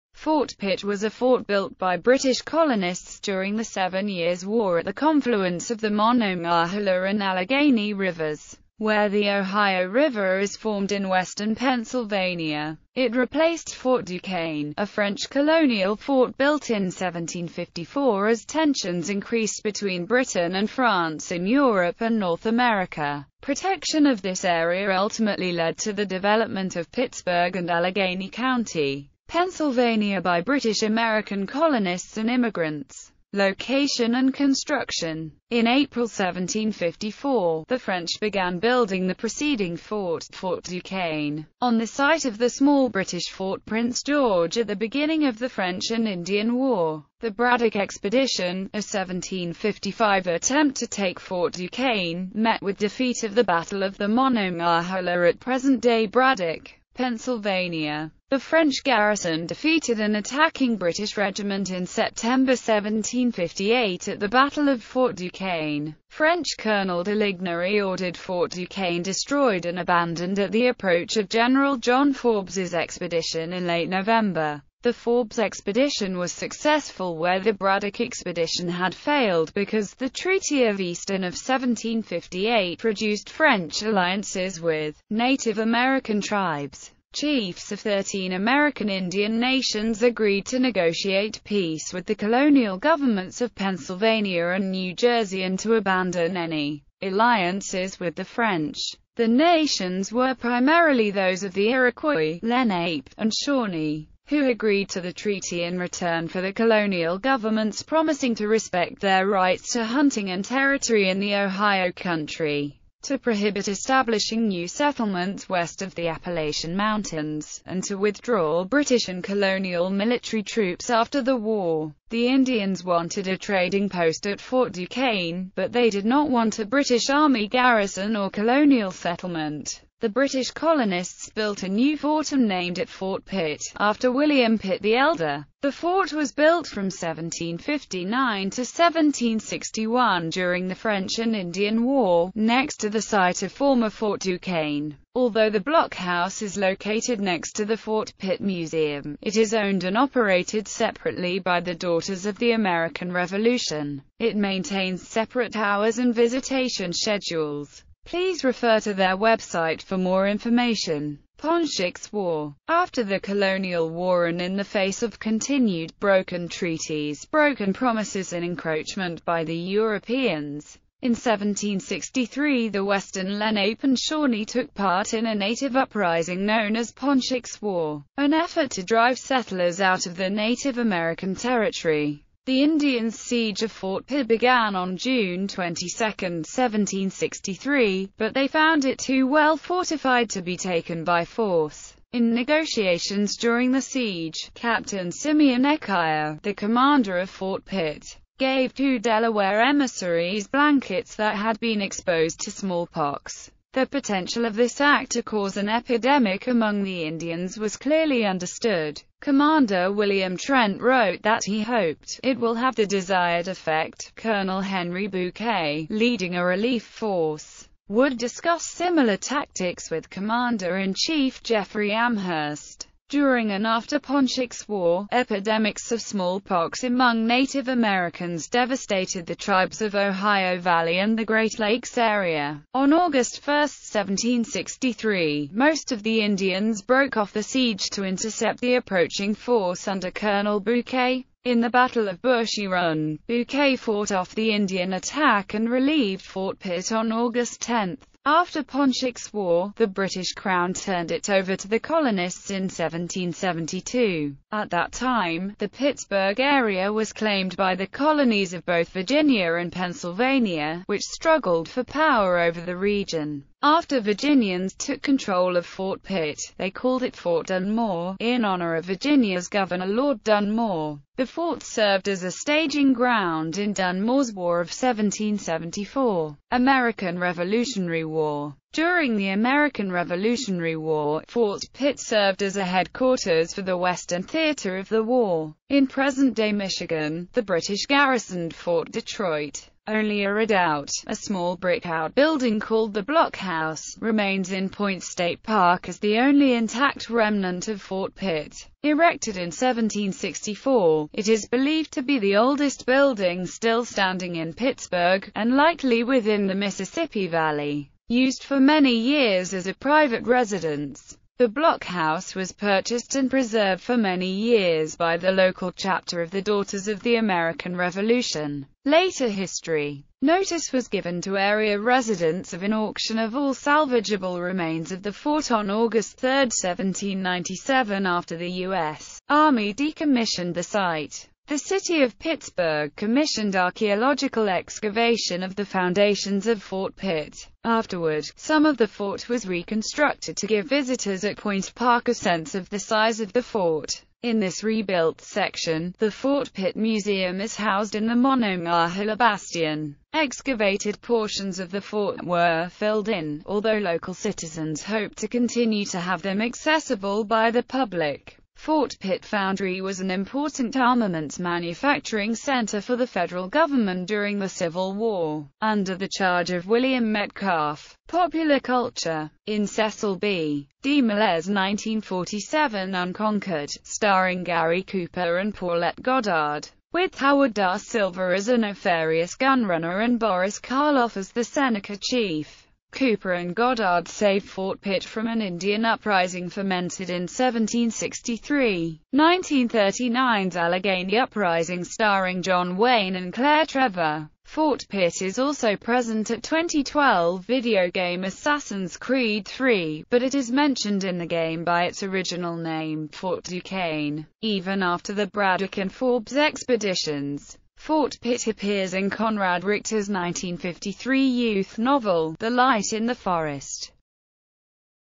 Fort Pitt (Pennsylvania)
Fort Pitt was a fort built by British colonists during the Seven Years' War at the confluence of the Monongahela and Allegheny rivers, where the Ohio River is formed in western Pennsylvania.

Image-Copyright-Info